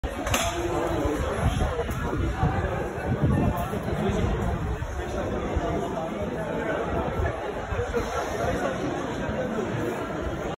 Keen on Istanbul's Colorful Spice Market? Traveler's Gem!
Explore the vibrant Spice Market in Istanbul, a must-see for travelers! Discover an array of spices, aromas, and cultural experiences.